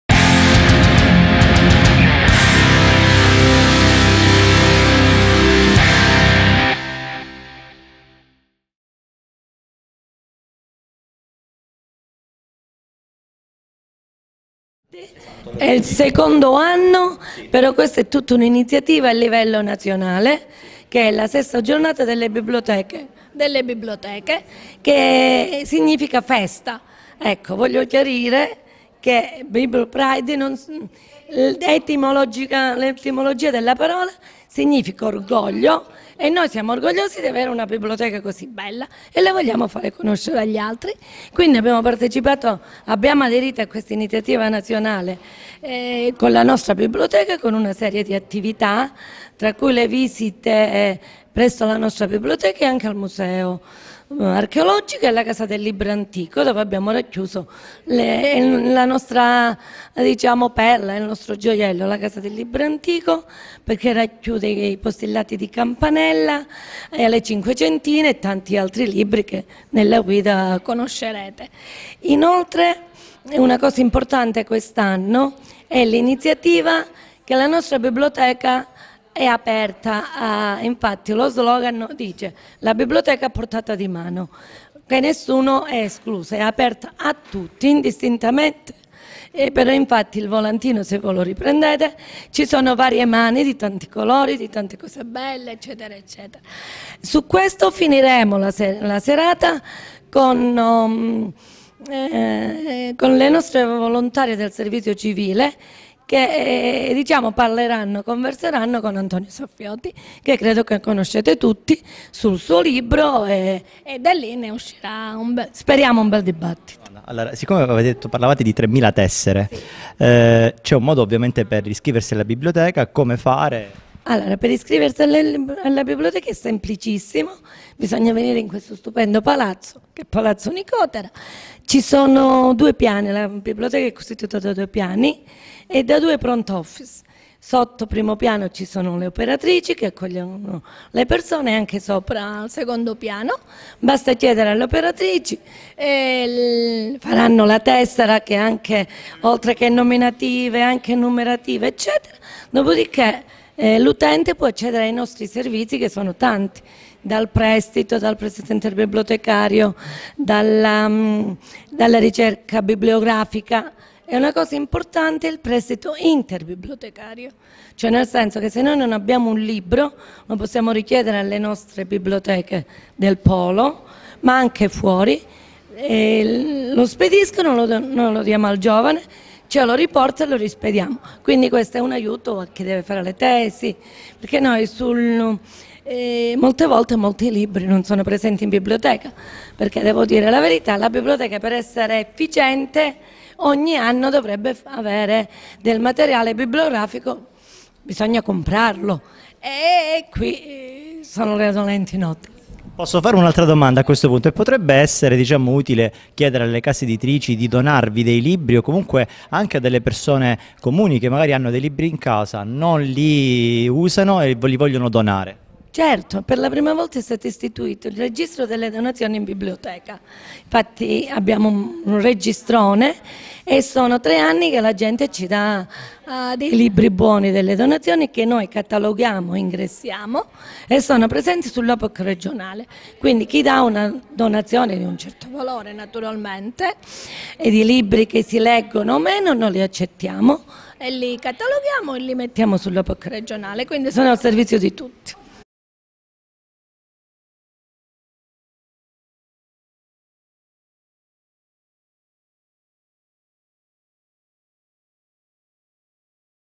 Biblio Pride 2017 - Intervista alla Prof.ssa Claudia Brunetti - Lamezia Terme (Cz)
Intervista alla Prof.ssa Claudia Brunetti, responsabile Biblioteca Comunale "Oreste Borrello" di Lamezia Terme.

BiblioPride 2017 - Giornata nazionale delle biblioteche

La biblioteca a portata di mano - 28 settembre 2017 a Lamezia Terme (Catanzaro)

Seconda edizione a Lamezia Terme:
- Inizio presso Palazzo Nicotera-Severisio.
- Visite guidate presso la Biblioteca Oreste Borrello, Casa del Libro Antico, Museo Archeologico con finalità di promozione culturale e territoriale
- Incontri di crescita personale approfondendo il tema dell'accoglienza e accessibilità.

Organizzato dal Comune di Lamezia Terme, in collaborazione con: Biblioteca comunale, Piazza Campanella , Lamezia Terme.